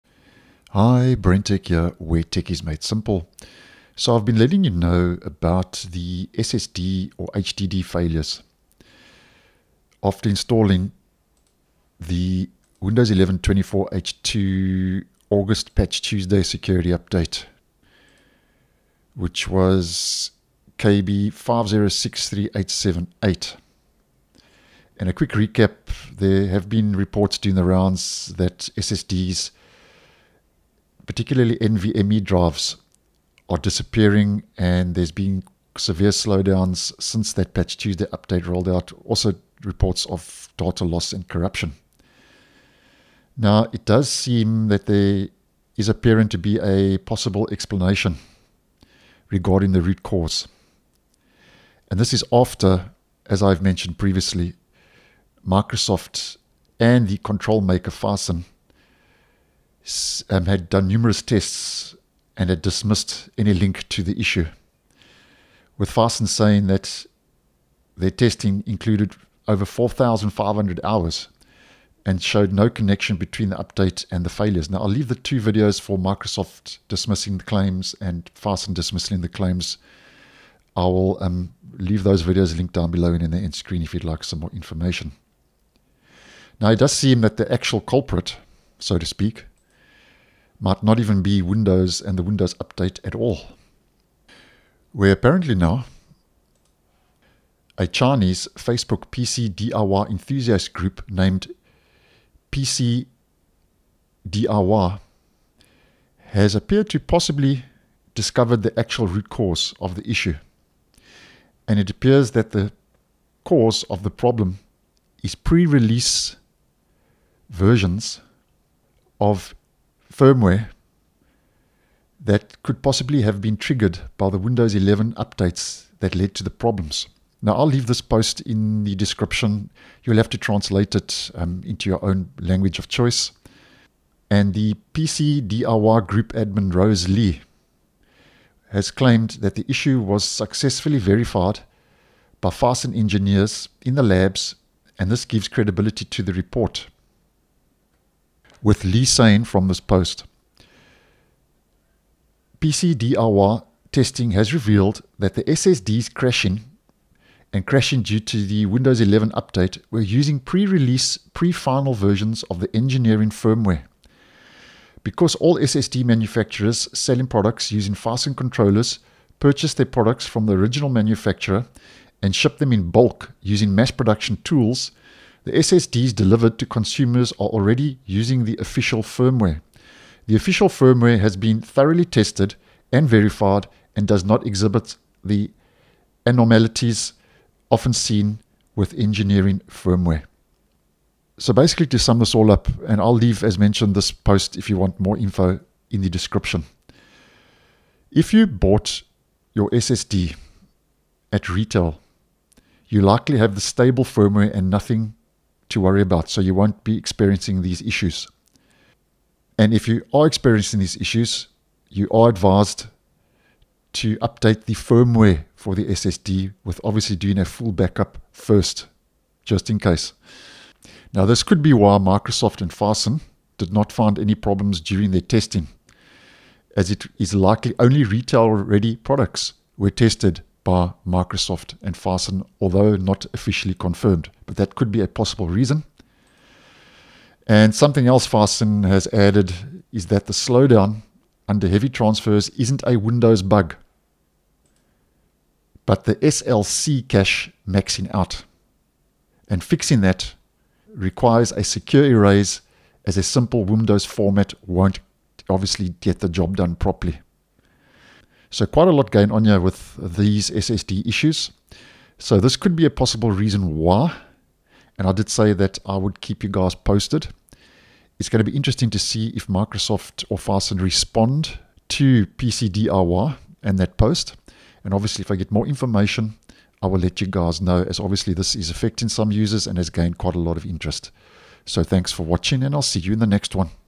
Here's Possibly Why Windows 11 is Breaking SSDs!
As reported by Facebook group PCDIY, the issue may not be Windows at all, but with pre-release firmware on certain SSDs
Windows 11 24h2 is BREAKING SSDs: Phison Says NO Corruption Could be Reproduced! (Most NOT Affected)